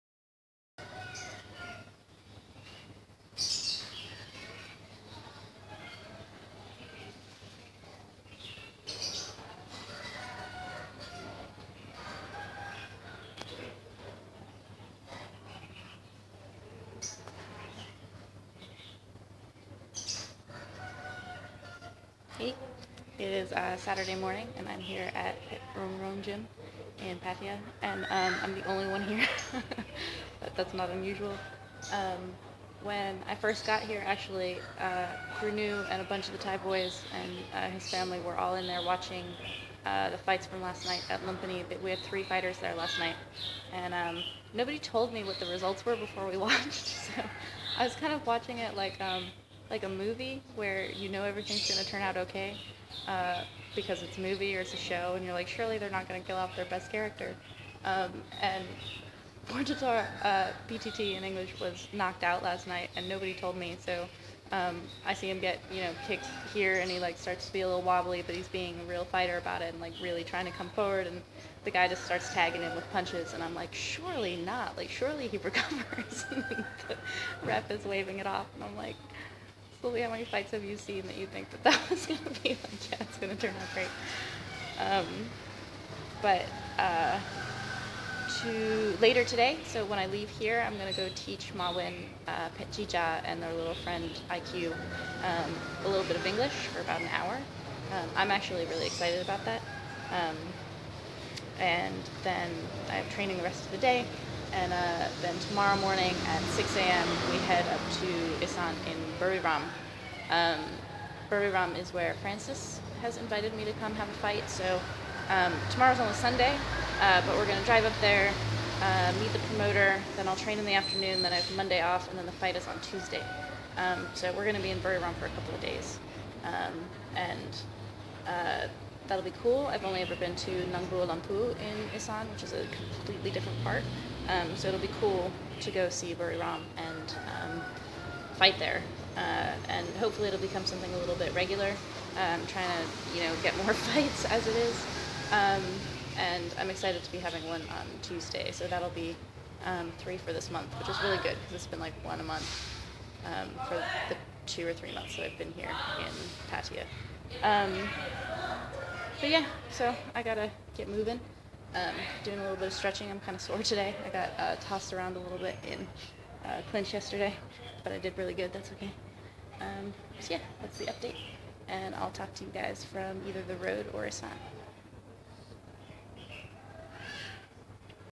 Vlog #108 - Teaching Phetjee Jaa English - Headed to Buriram to Fight
Only one here, watching last nighta fights, teaching english to the kids in an hour, Buriram tomorrow.